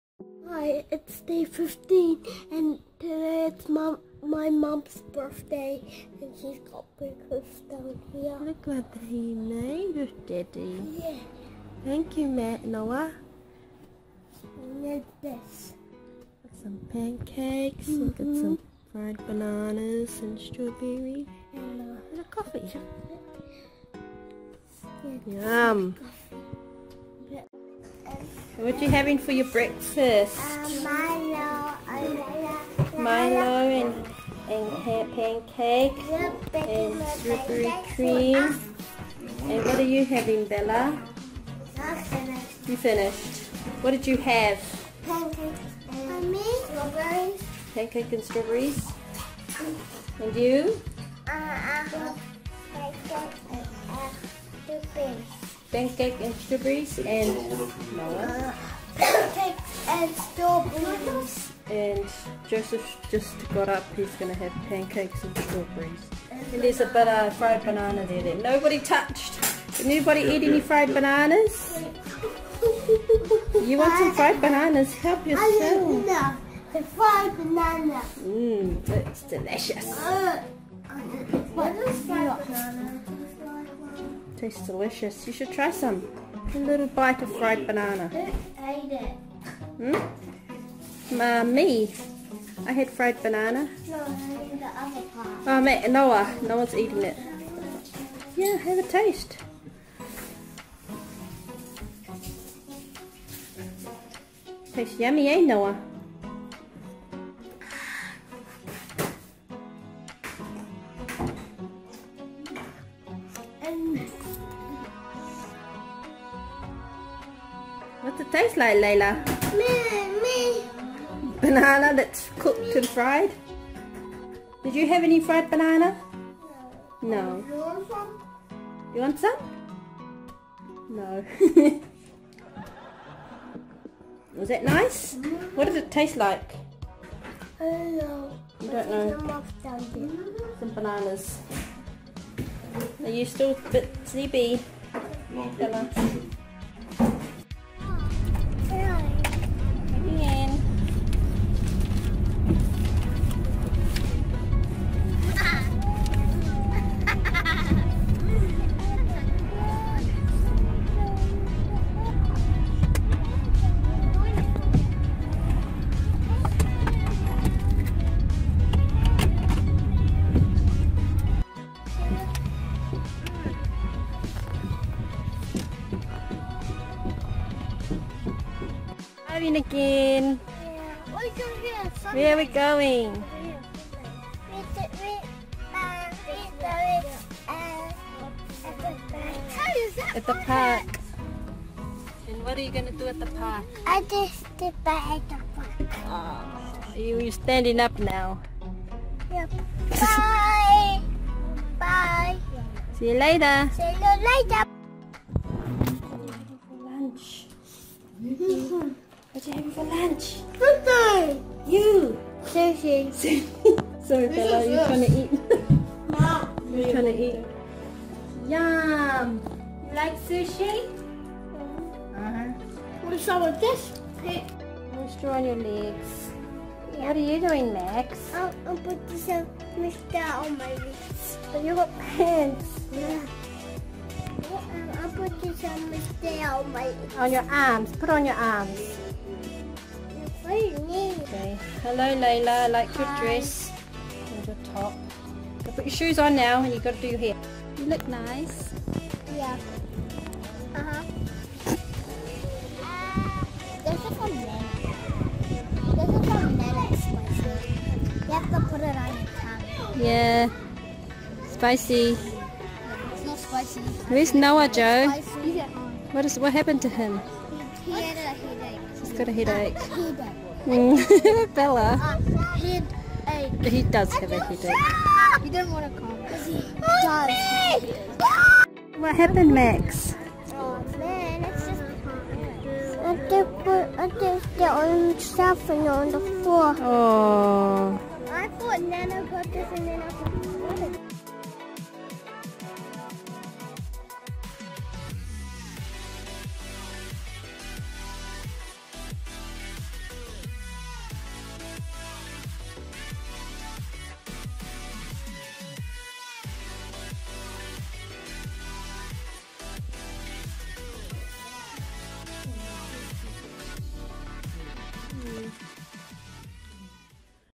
♥ "HAPPY BIRTHDAY MUM!" ♥ [Toys, Family Fun & Games by 7]
On this day it was Mum's birthday. Noah got up early with the help of Dad and made breakfast for her. Then it was off to church, poor Noah was feeling sick. Then a lovely relaxing dinner and Red Velvet Cake! All in the comfort of home! The perfect way to spend a birthday. :) Feeling happy and loved. Thanks everyone. xx.

♥ Toys, Family Fun & Games by 7♥